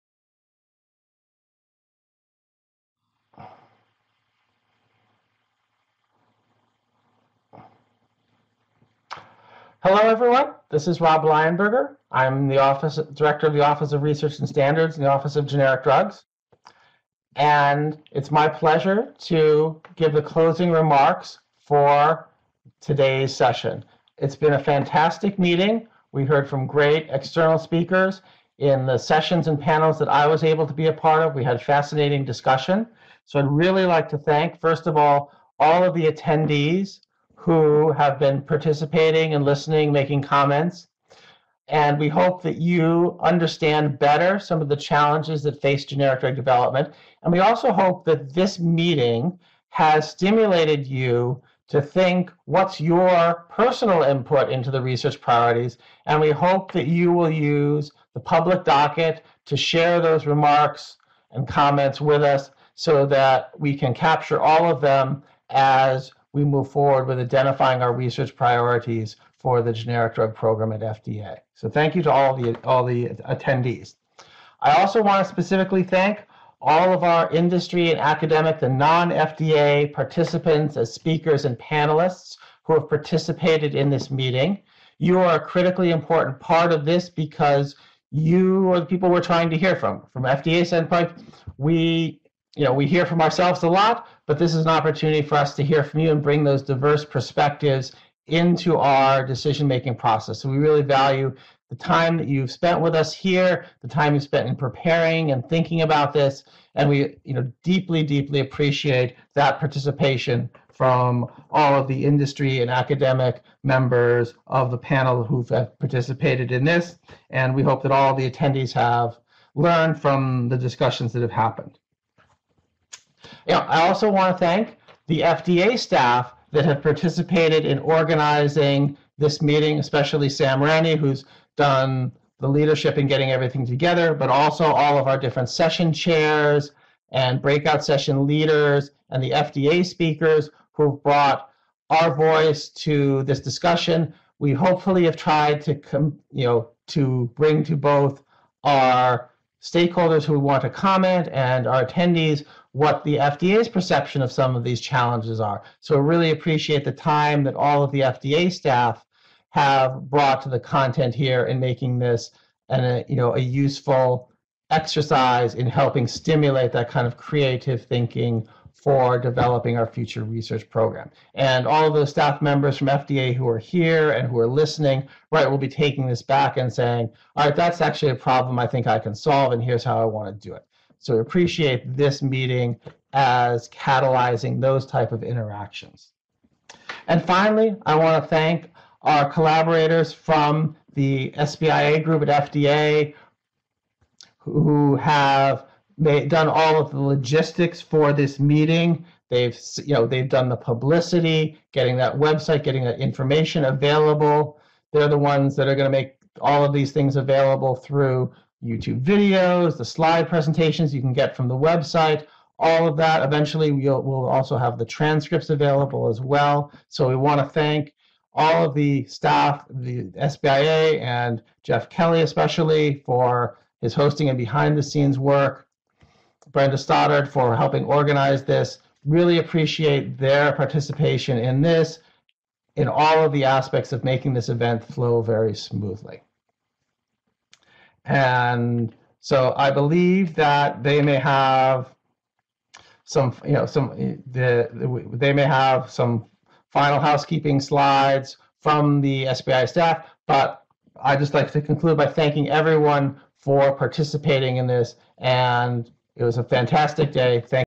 Closing Remarks
Robert Lionberger, Ph.D., director, Office of Research and Standards in the Office of Generic Drugs at CDER provides closing remarks.

FDA CDER’s Small Business and Industry Assistance (SBIA) educates and provides assistance in understanding the regulatory aspects of human drug products & clinical research.